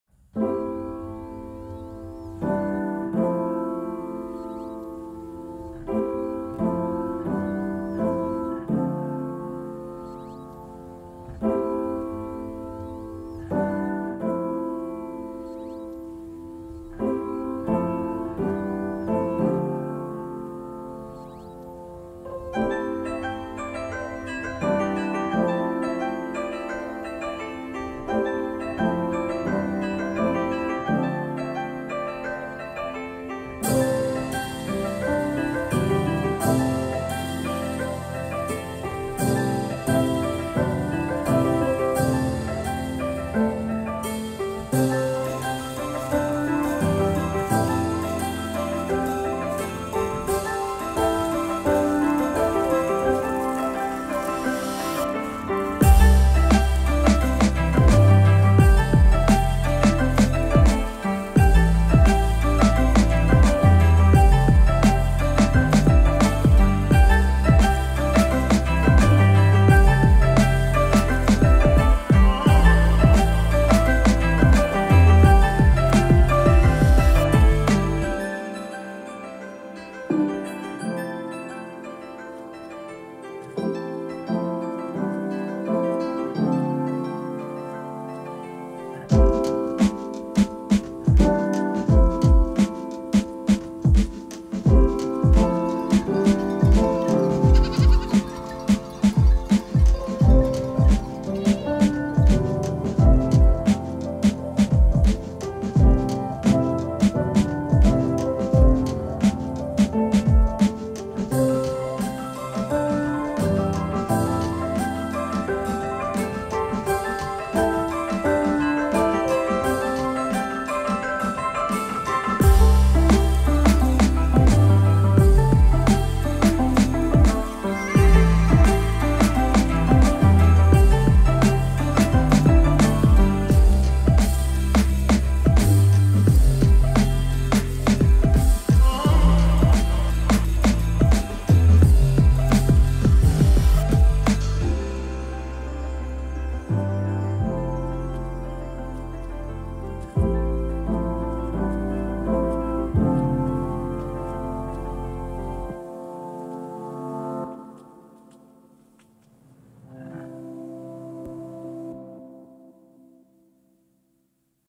Gudrun Sjödén goes to La Gomera to shoot Autumn 2018 collection
Our team went to La Gomera to shoot the beautiful Autumn 2018 collection. The collection is inspired by the forest, ridges and everything inbetween. Don't miss out on the collection